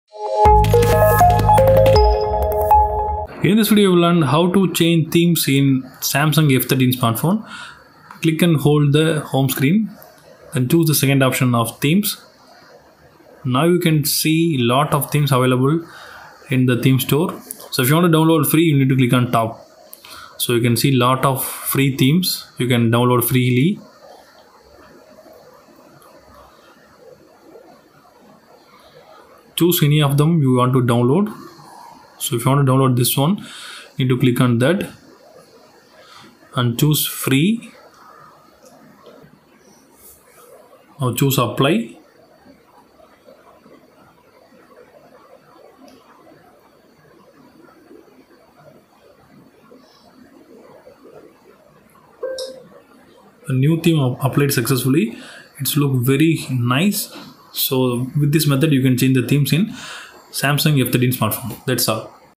How to Change Themes in Samsung F13
Hey Friends  ! Checkout this new video of How to Change Themes in Samsung F13